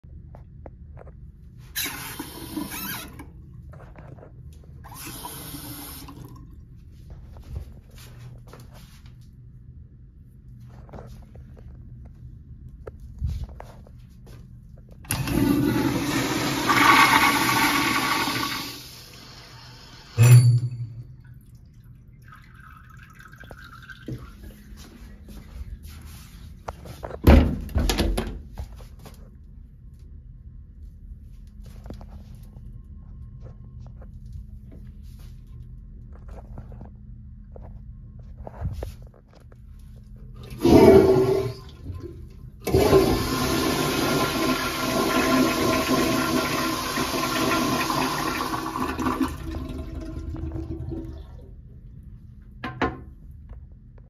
WSU Multi-User Unisex Restroom (Webster Physical Building Of Astronomy)
11th floor of the same building as last video. This is super duper weird but nice either way!

Fixture list:
-1 1969 American Standard Jetbrook urinal
-1 1969 American Standard Glenco toilet